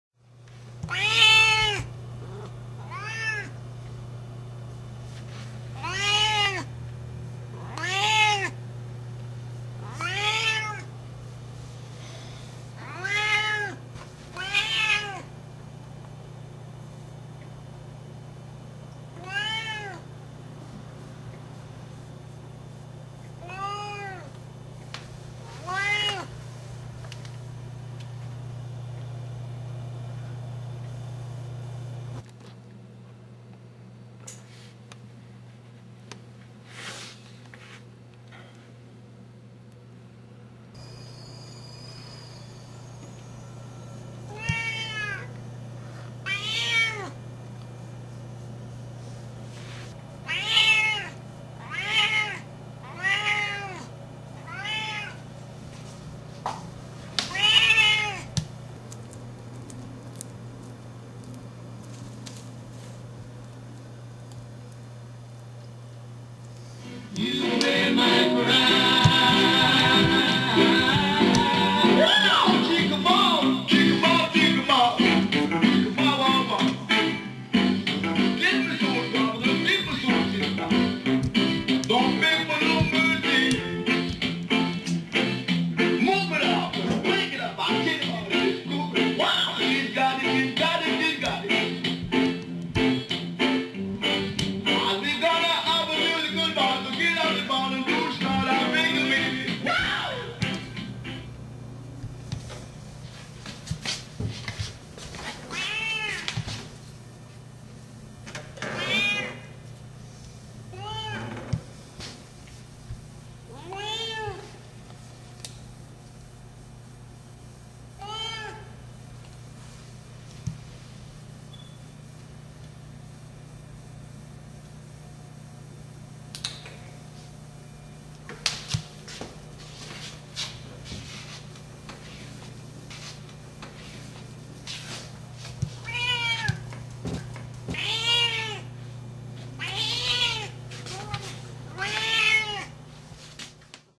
Loud Siamese Cat - Part 2 - Sienna is looking for love
R.I.P. Sienna. You were always loud and beautiful. Will miss you forever, love you.

Sienna died May 26, 2013 at the age of 17 years and 2 months from liver cancer. She was 12 years old in this video.

Born to a Siamese father and Himalayan / Persian mother,
Sienna keeps looking at her heart shaped plate...